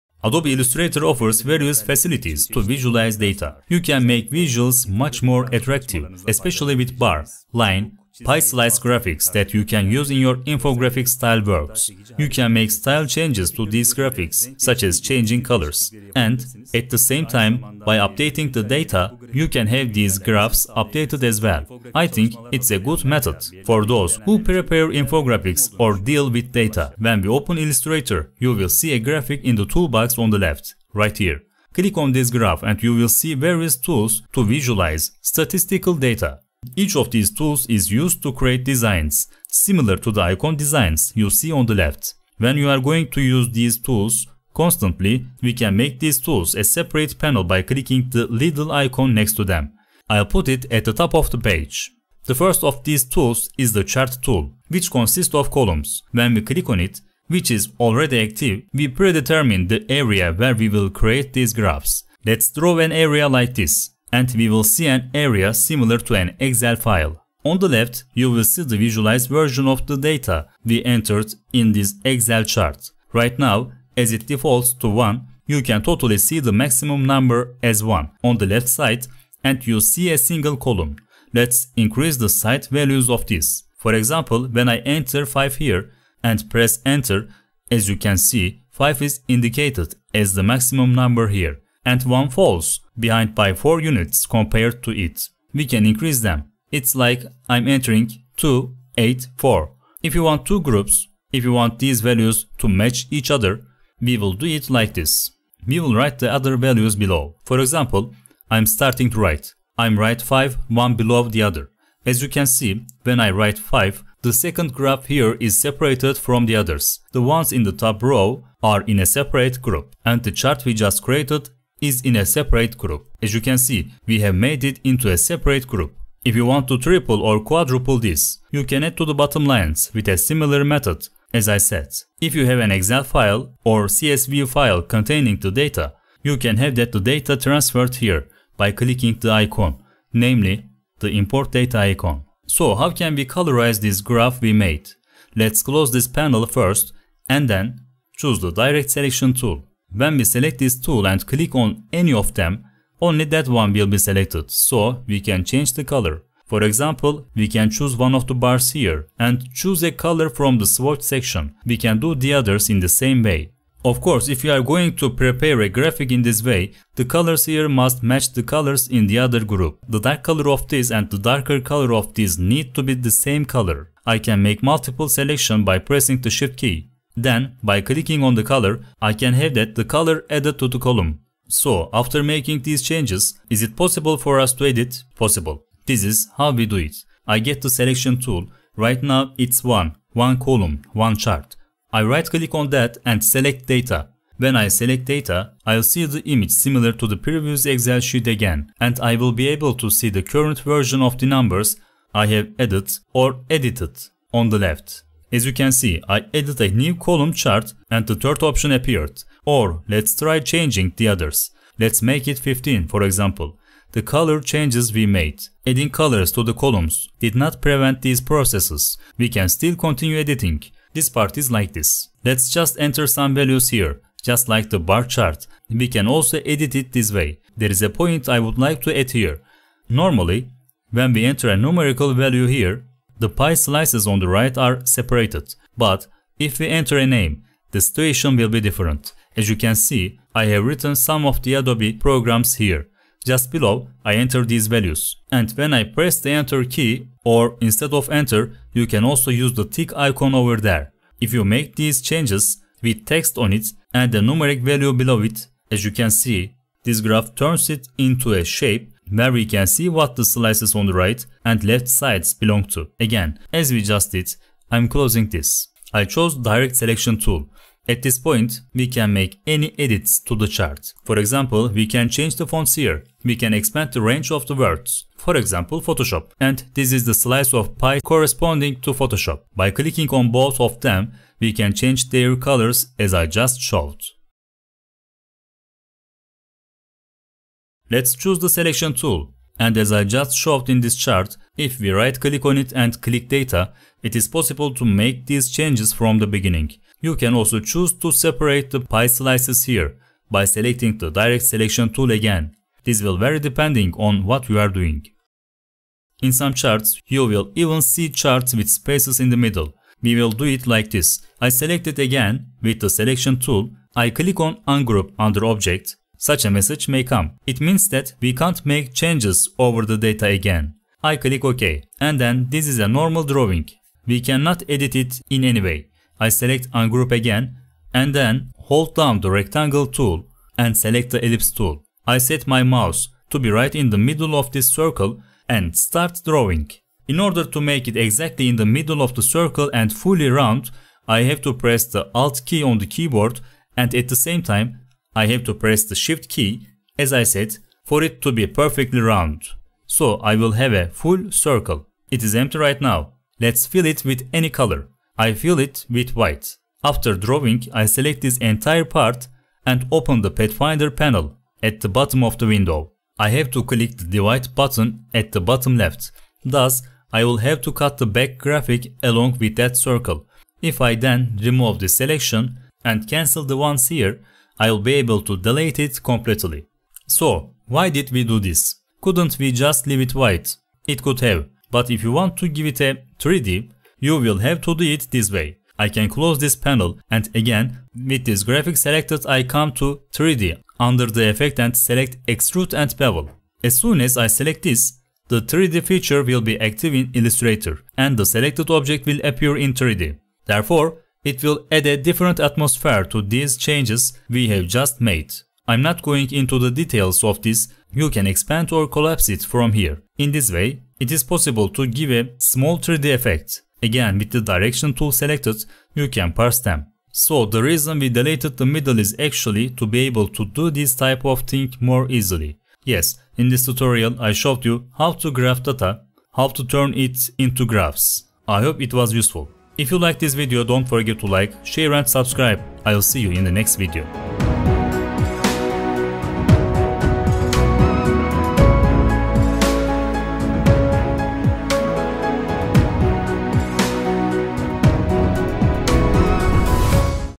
DATA VISUALIZATION IS IMPORTANT! Make Charts & Graphs for Beginners in Adobe Illustrator
In this Adobe Illustrator tutorial, I'll show you how to create custom graph charts for your infographic and data visualization designs. You can make visuals much more attractive with bar, line, pie slice graph charts.